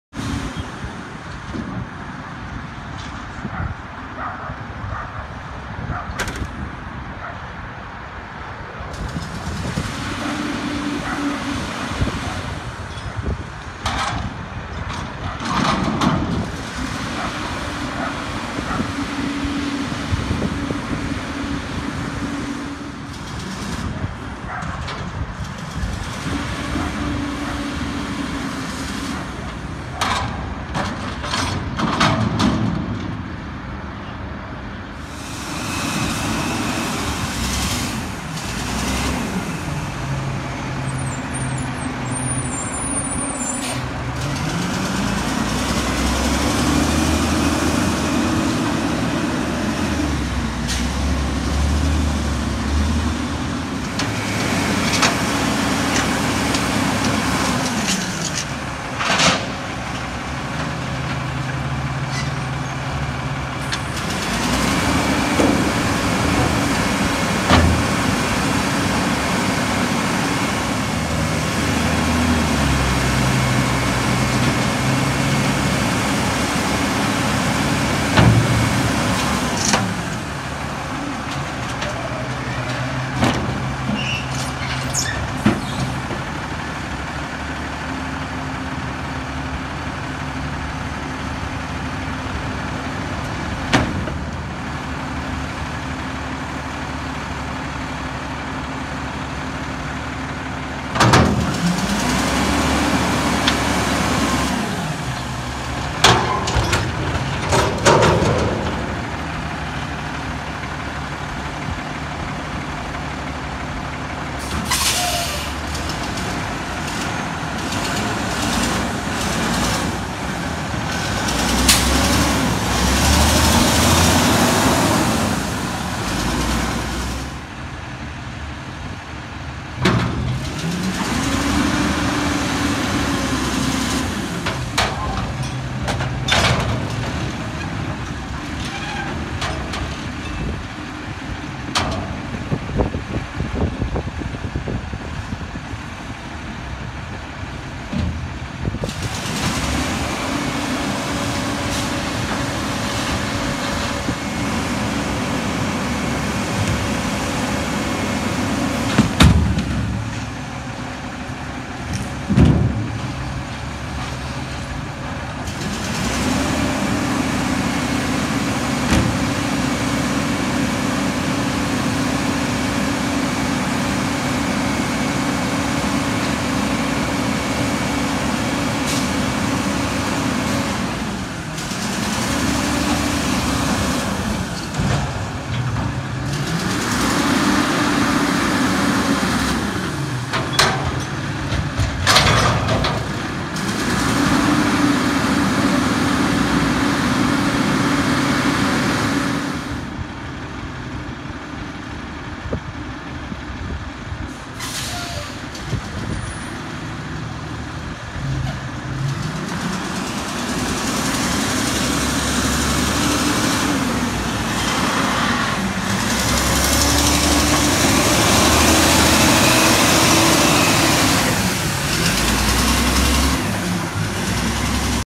Athens Services front loader Trash Truck #370 Autocar Xpeditor Part 7
That truck is back since a month being in my hood and did good